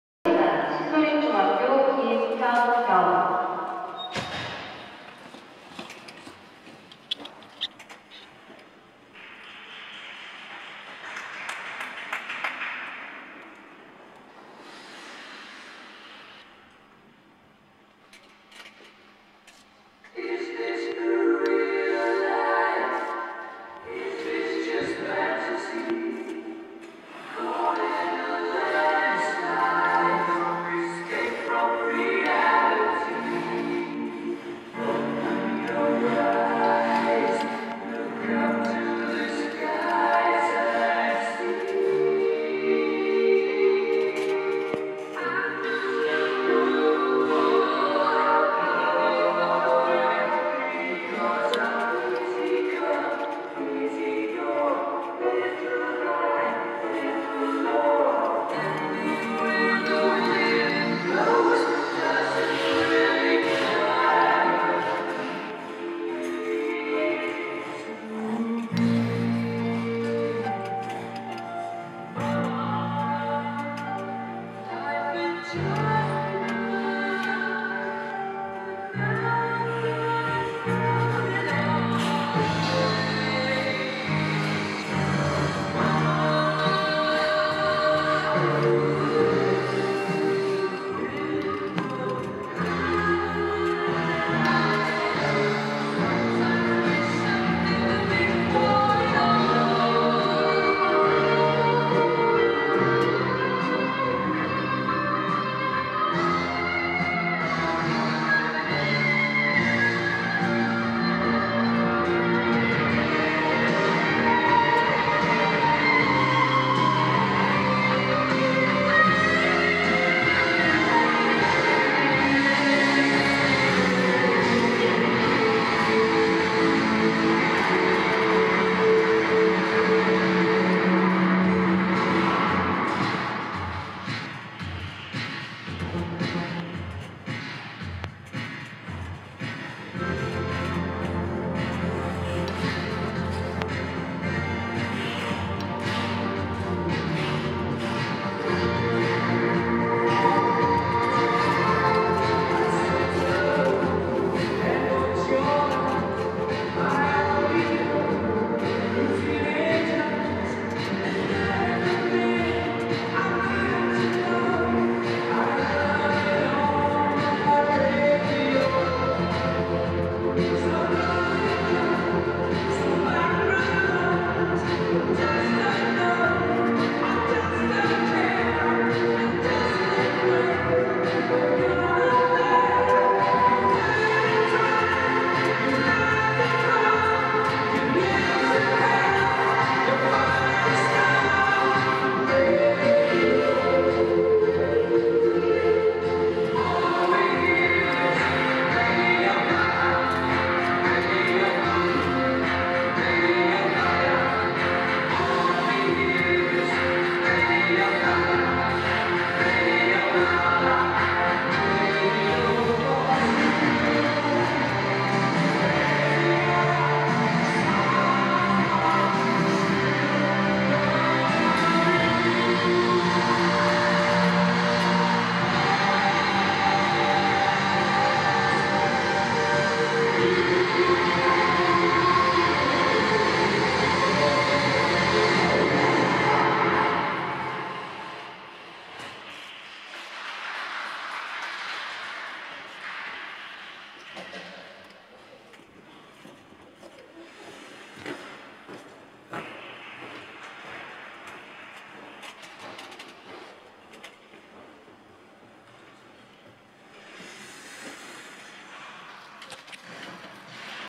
김현겸 Hyungyeom KIM | FS 1 | 2019-12-02 서울시장배 및 동계체전 예선 | 신도림중 1 | 6급
2019 서울특별시장배 빙상경기대회(피겨)겸 제101회 전국동계체육대회 서울시 예선대회 | 2일차 | 광운대학교 동해문화예술관 아이스링크 |
(Sep 02. 2019) Seoul City Mayor Cup 2019 | Day2 | 2019 101th (Korea) National Winter Sports Festival. Seoul City Qualifire |
Kwangwoon University Ice Rink (B1, Donghae Cultural Art Center) | Republic of Korea (South Korea)

B Ladies Elementary-Middle School, University, Adult (Junior) | Free Skating |
A Ladies Middle-High School, University (Senior) | Free Skating |